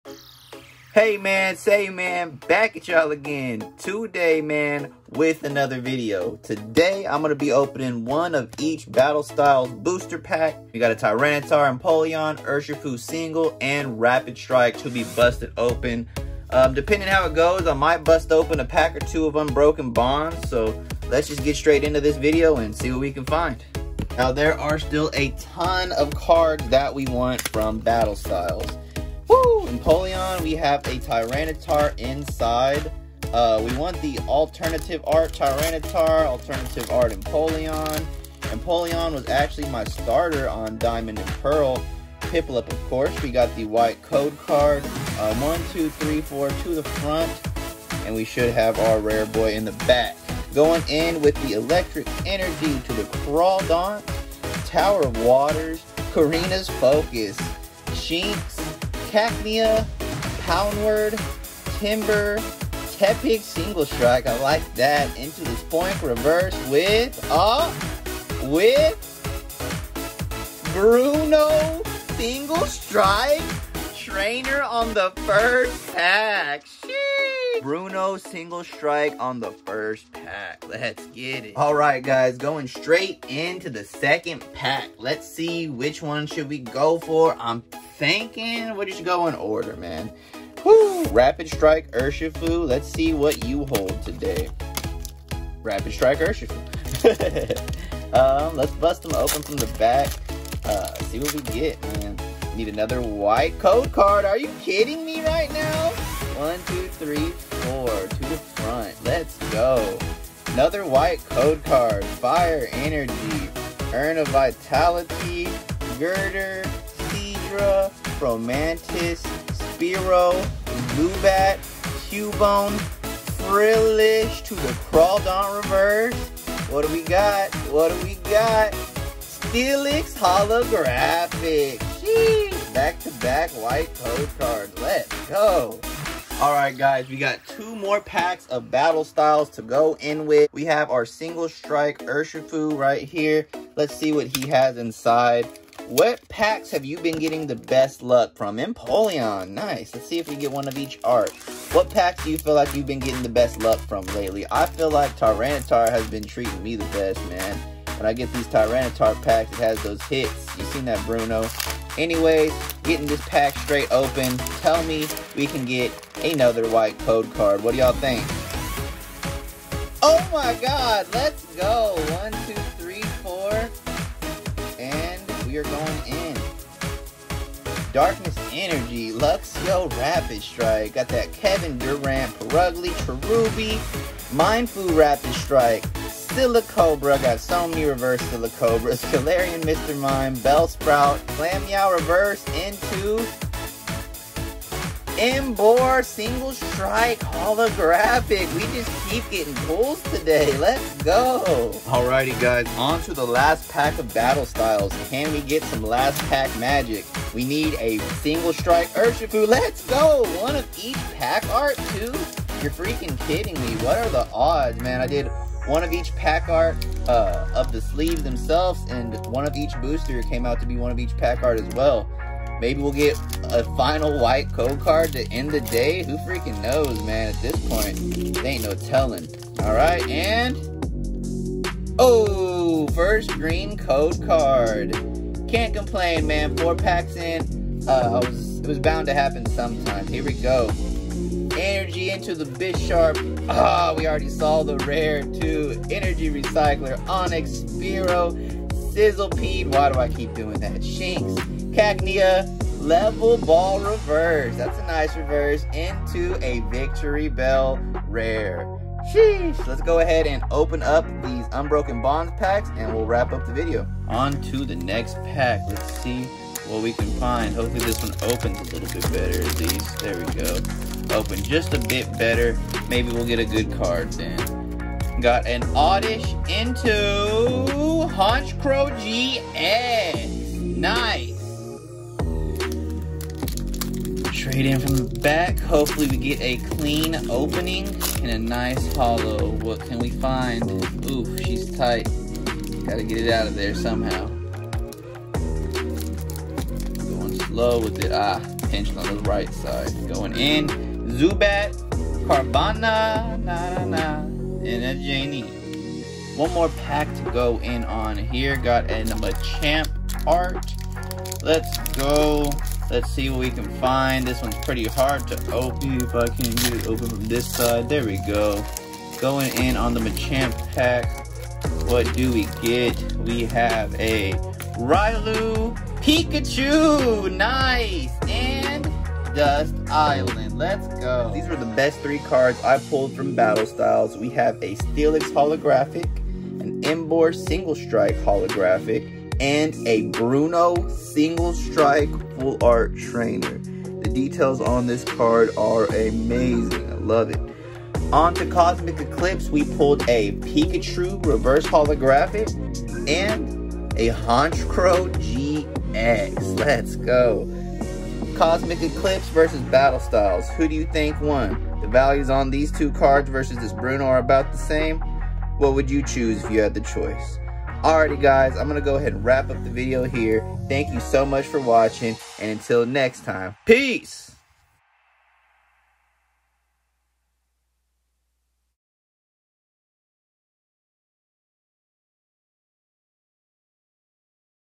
Pokémon Battle Styles Full Art Pulled!
In this video I open some battle styles booster packs. The video went so well I decided to go ahead and open some unbroken bonds to. I pulled a nice full art and a GX card. Any questions comments or concerns let me know down below and I hope you have a wonderful day.